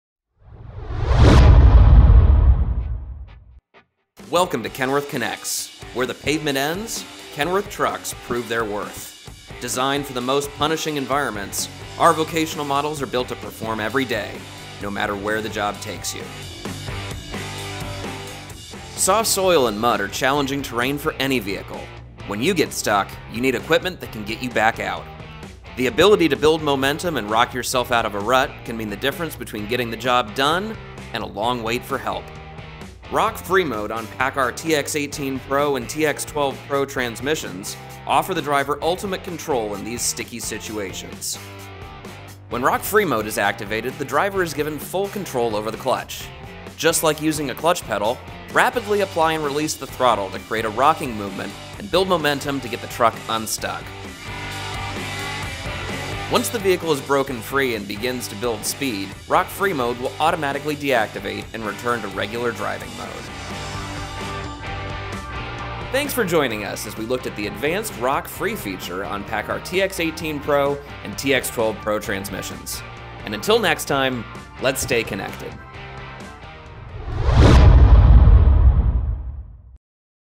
Kenworth PACCAR TX-18 PRO Transmission Rock-Free Mode
What if your truck could get you unstuck? Join Kenworth Connects as we look at the Rock Free feature for the PACCAR TX-12 PRO and TX-18 PRO transmissions. Perfect for soft soil and mud, this feature can help get you out of a bind!